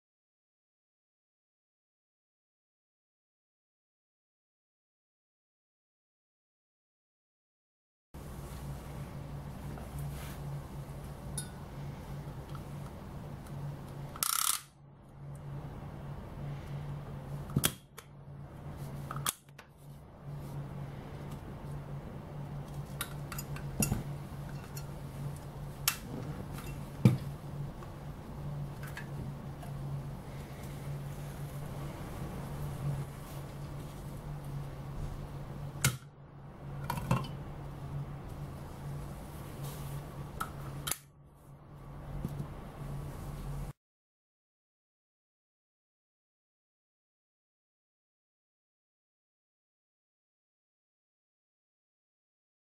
EasiCap Bottle Tag Operation Guide
A basic instructional video showing the use and operation of the EasiCap Bottle Tag available from Easitag.